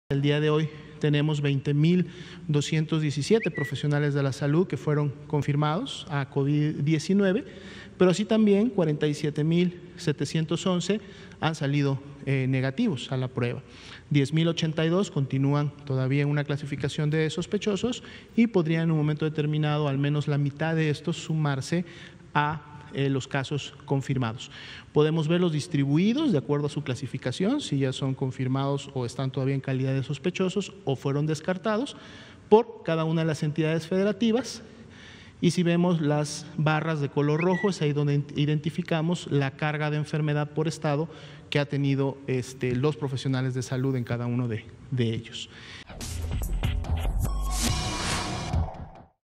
Suman 20 mil 217 profesionales de la salud positivos a COVID-19
José Luis Alomía Zegarra, dir. gral. Epidemiología, dio a conocer que a la fecha son 20,217 profesionales de la salud los que han dado positivo a COVID-19, mientras que  47,711 dieron negativo, y 10,082 se encuentran sospechosos.